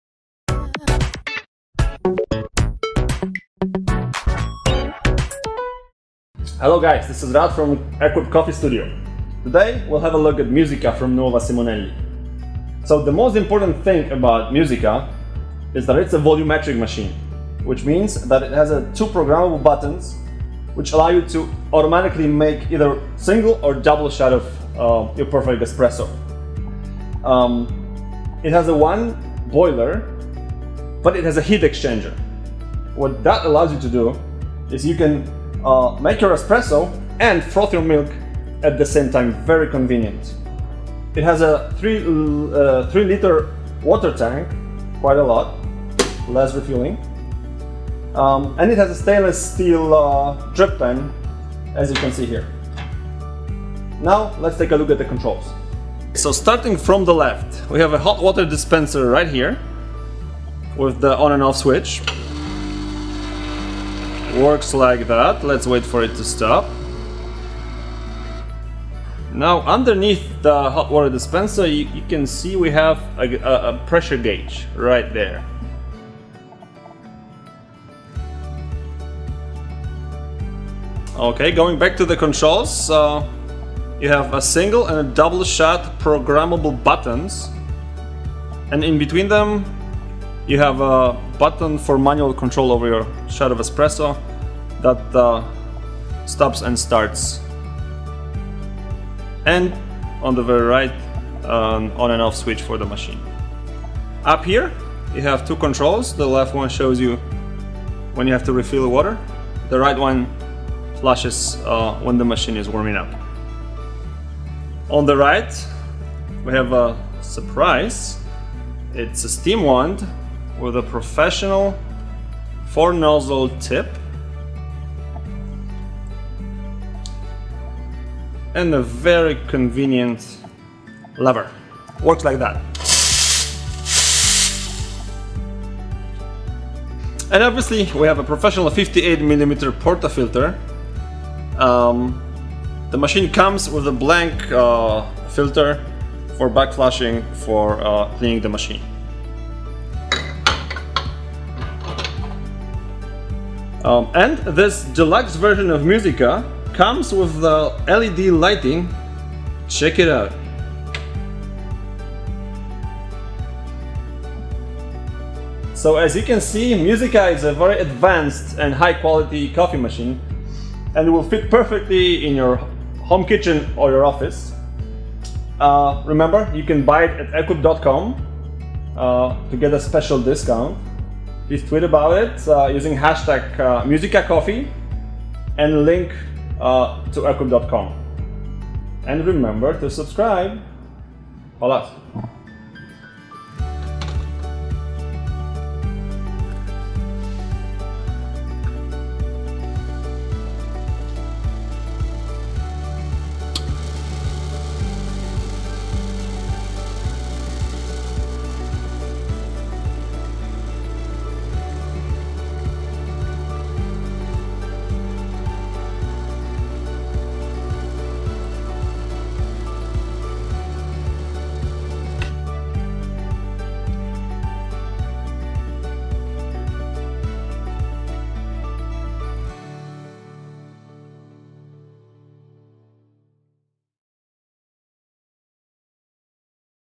Nuova Simonelli Musica review - Ekuep.com
Musica might be just the perfect answer if you're looking for a very solid espresso machine for your home or office.

It's volumetric, and that means very simple operation.

It's also a top quality product, producing a perfect cup of espresso every time.